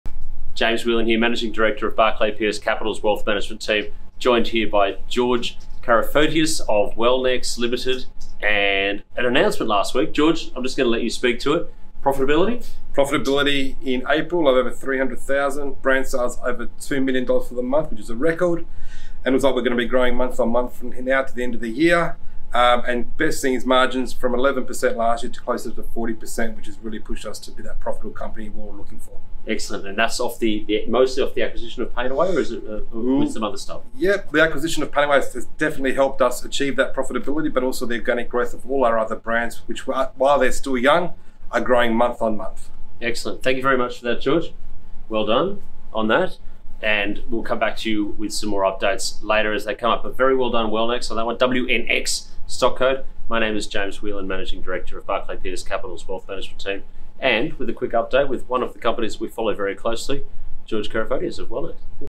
Interview with George Karafotias of Wellenx Life (ASX:WNX)
BPC is excited to share an exclusive interview with George Karafotias, CEO of Wellnex Life(ASX: WNX).

Key Highlights:

1. Wellnex Life achieved record profitability, with over $2 million in branded sales for April. Margins increased from 11% to close to 40%.

2. The acquisition of PainAway contributed to this profitability, along with the organic growth of other brands within the company, which are experiencing consistent month-on-month growth.

A huge congratulations to George Karafotias, Zack Bozinovski, the board and the team at WNX for reaching this exciting milestone!